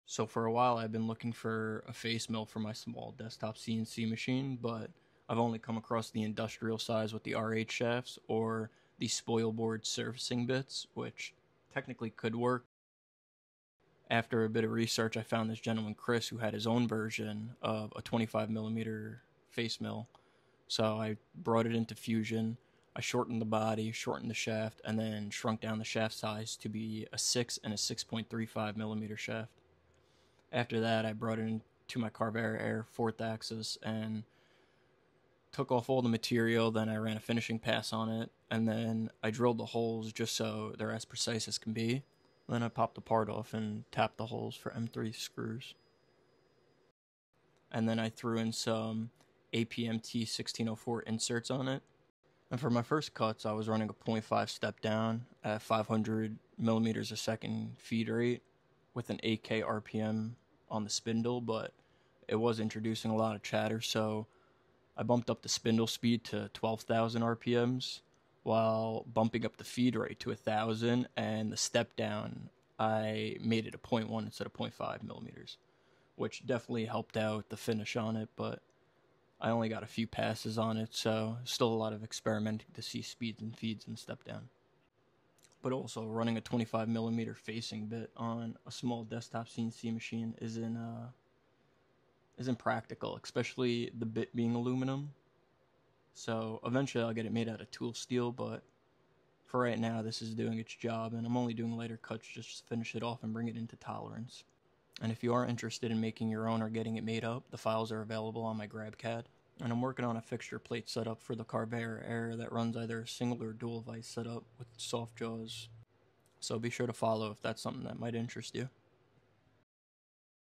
Desktop CNC 25mm Face-milling bit
25mm Face mill for small desktop machines using 6mm and 6.35mm shaft.
uses APMT1604 cutting Inserts.